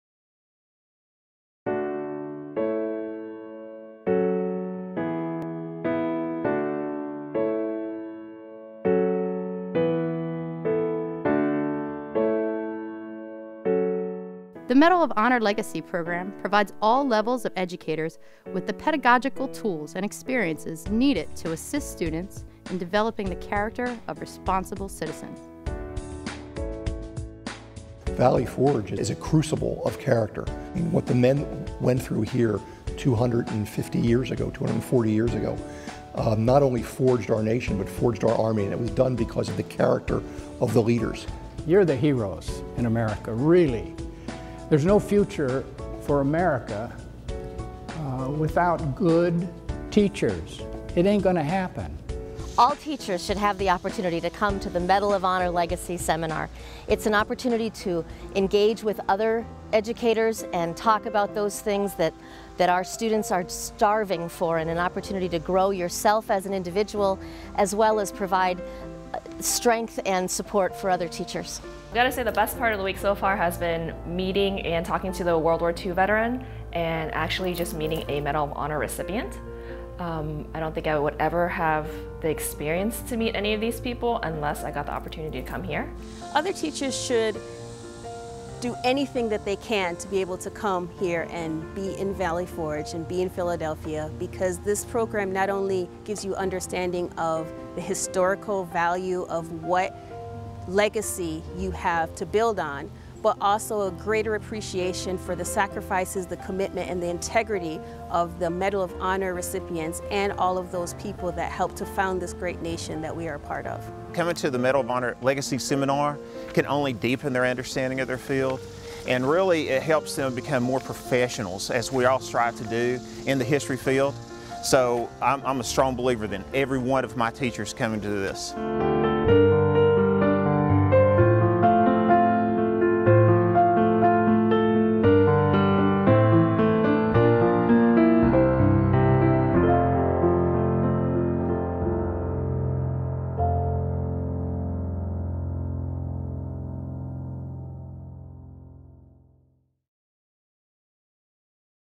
Medal of Honor Legacy Commercial - 2 minutes
Medal of Honor Legacy graduate seminar for teachers, a one week, residential professional development program focused on character development, American history, veterans, and the Medal of Honor.